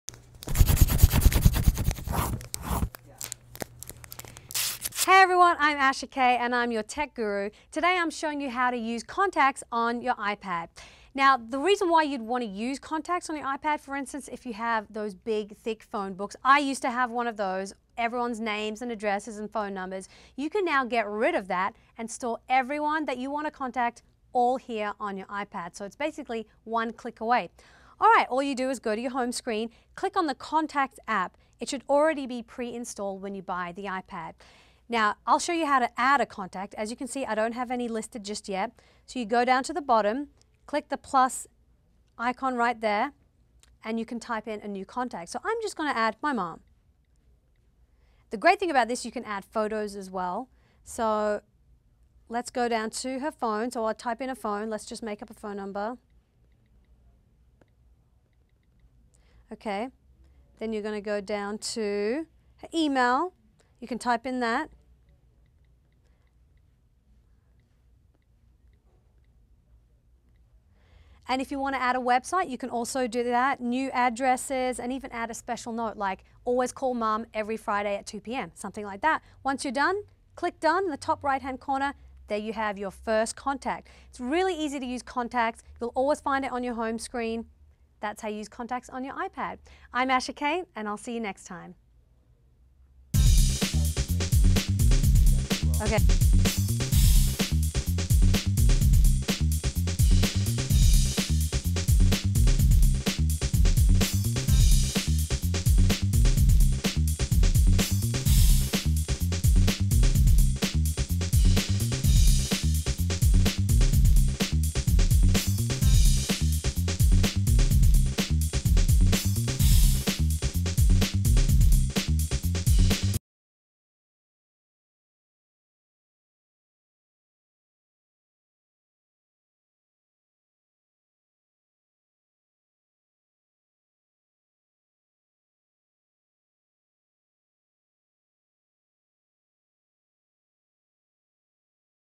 How to Add Contacts on The iPad
Mix your music! Learn GarageBand in 30 Days!

Watch and learn from our hostess Asha K. as she shows you the latest tips and tricks on one of the most popular electronic devices, the iPad.